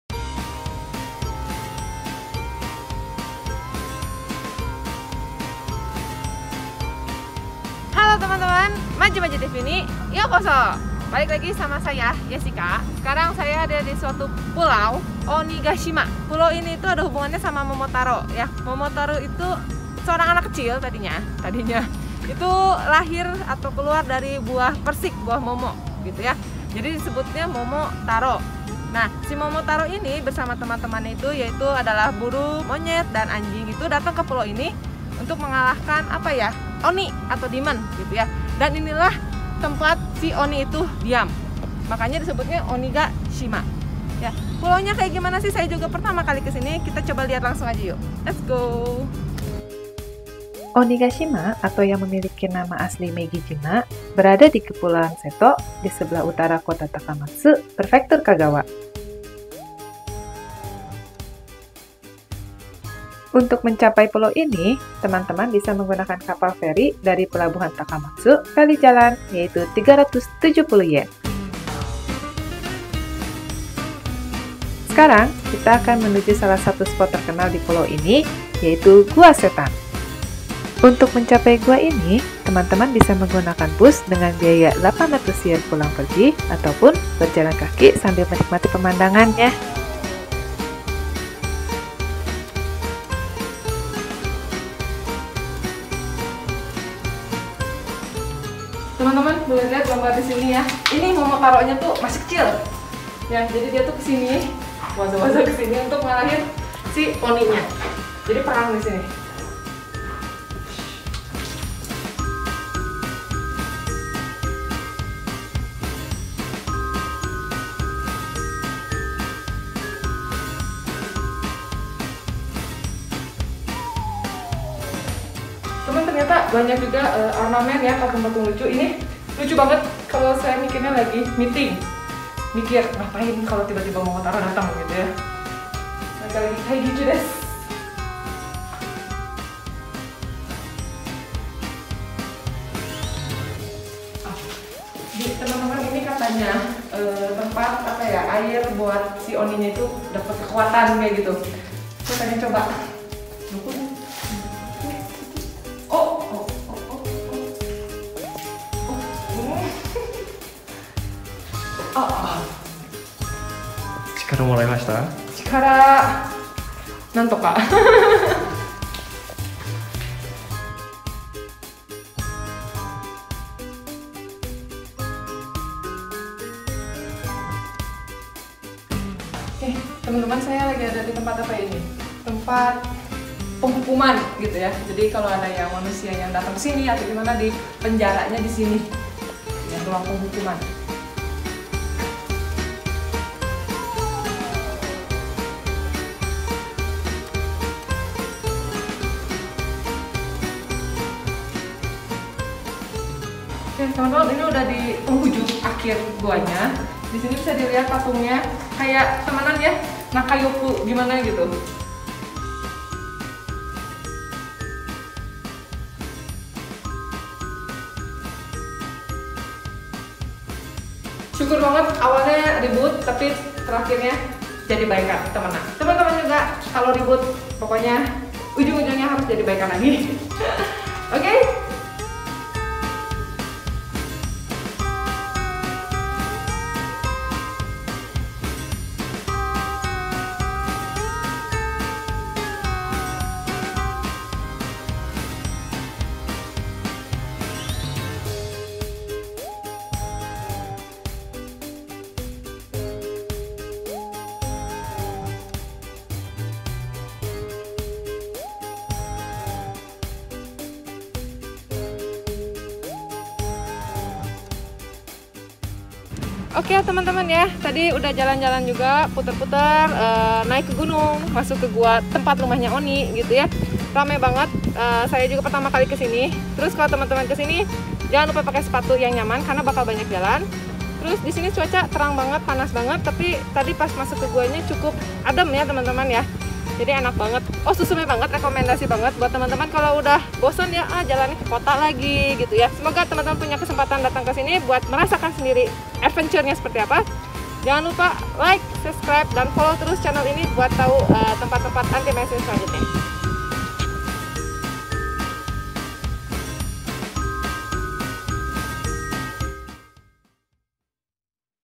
Onigashima, Tempat Momotaro vs Oni (Setan) | Majimaji TV
Temen-temen, di episode kali ini kami punya rekomendasi travel anti-mainstream buat temen-temen, khususnya menjelang Halloween akhir bulan ini! Yakni, Onigashima atau pulau Setan di Takamatsu! Lucu-lucu pokoknya!

Pulau ini juga punya kaitannya dengan Momotaro, yakni dongeng Jepang tentang anak yang lahir dari buah persik dan melawan setan. Nah, jadi Momotaro tuh perangnya disini temen-temen!

Ayo buruan cari tau cara pergi ke sini, mumpung Halloween masih ada beberapa hari lagi!

Untuk menuju ke tempat ini, teman-teman bisa berangkat dai Takamatsu-port (dekat Takamatsu Staion di Kagawa Perfektur). Lalu temen-temen beli lah tiket menuju ke "Megishima 女木島".

Jangan lupa selfie sama Oni-san ya!